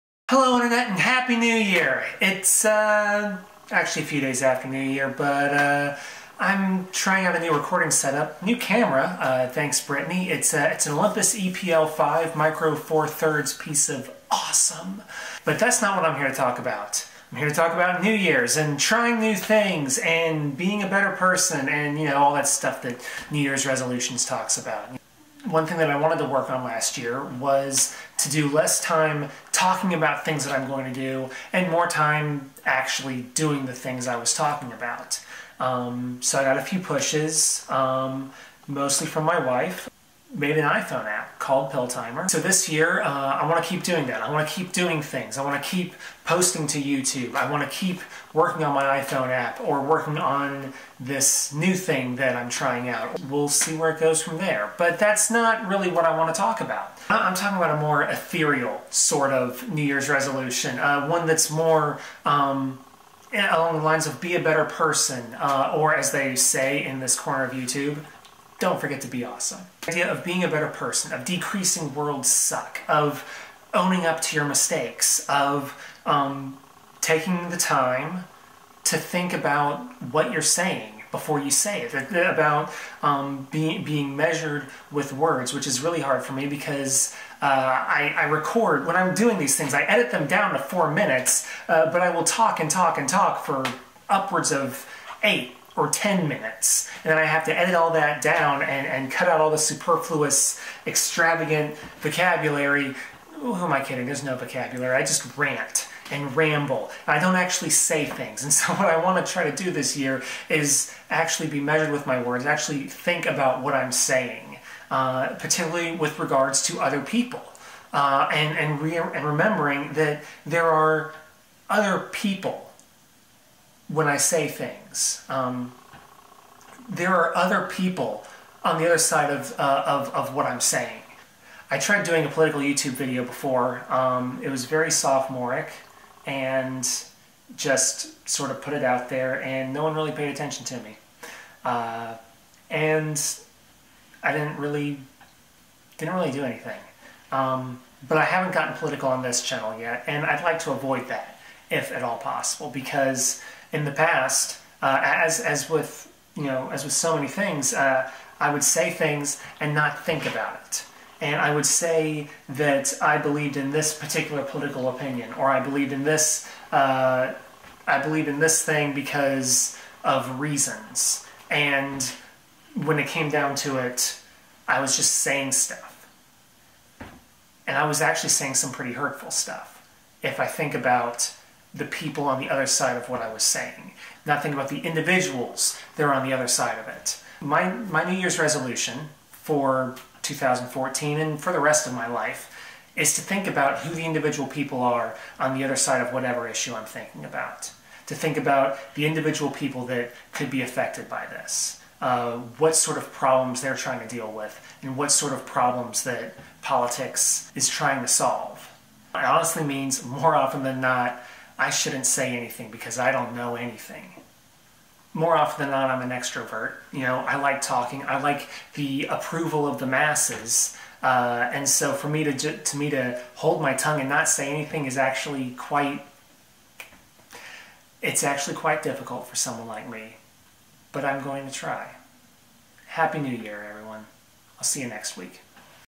On New Years
In which I ramble on about new years' resolutions and wanting to be a better person (in a DFTBA kind of sense). Welcome to 2014!